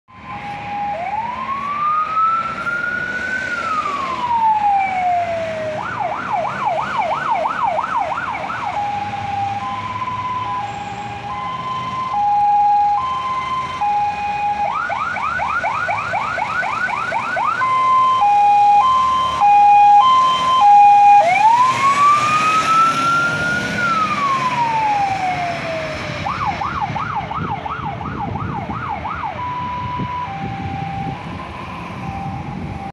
[ALARMOWO] OSP Kamionki, 339[P]52 do monitoringu ppoż
SiŚ:🚨

OSP Kórnik:🚒🚒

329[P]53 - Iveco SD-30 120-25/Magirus
329[P]51 - GBA 3/16 Volvo FL280/BoCar

OSP Kamionki:🚒

339[P]52 - GCBA 5/32 MAN TGM 18.340/BoCar

JRG 6 Poznań Krzesiny:🚒🚒

306[P]21 - GBA 3/16 Mercedes-Benz Atego 1530/BoCar
306[P]25 - GCBA 5/35 MAN TGM 18.340/Stolarczyk